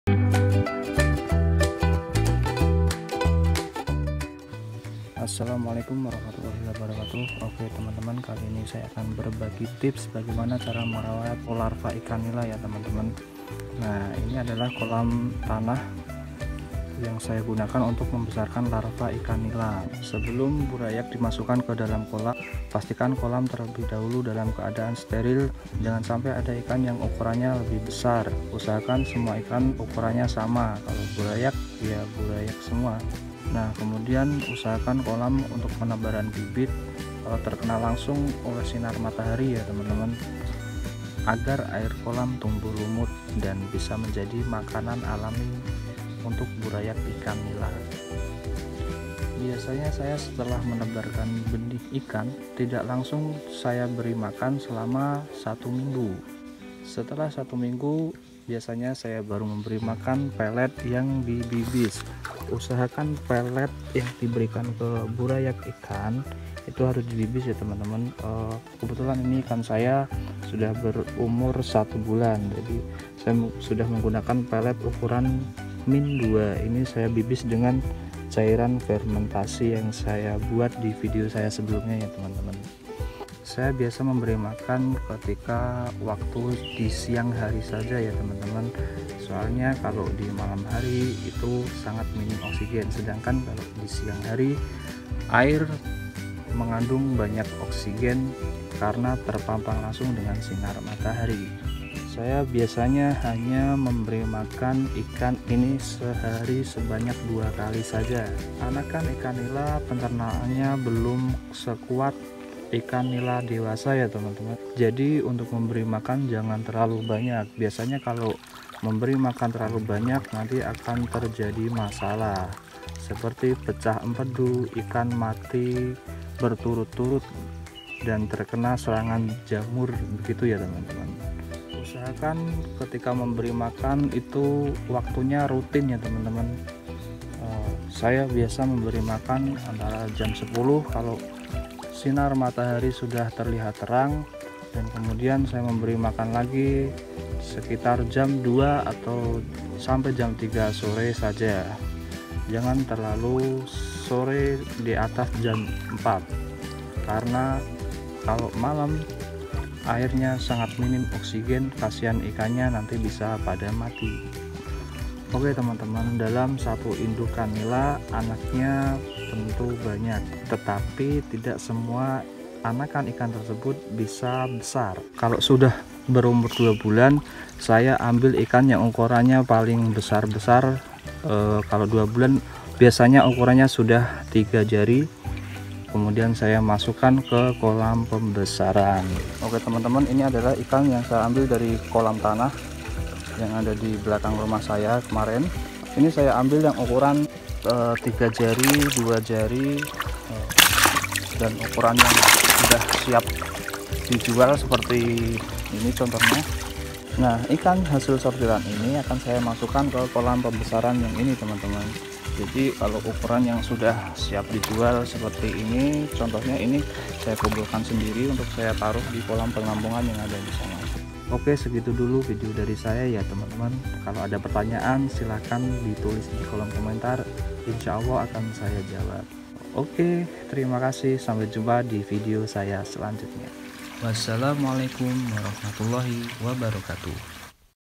CARA SUKSES MERAWAT LARVA IKAN NILA🐟 Di kolam tanah sampai masuk ke kolam PEMBESARAN 100% Berhasil
Oke gais, di video kali ini sy menceritakan pengalaman saya sukses merawat larva nila sampai masuk ke kolam pembesaran sistem RAS.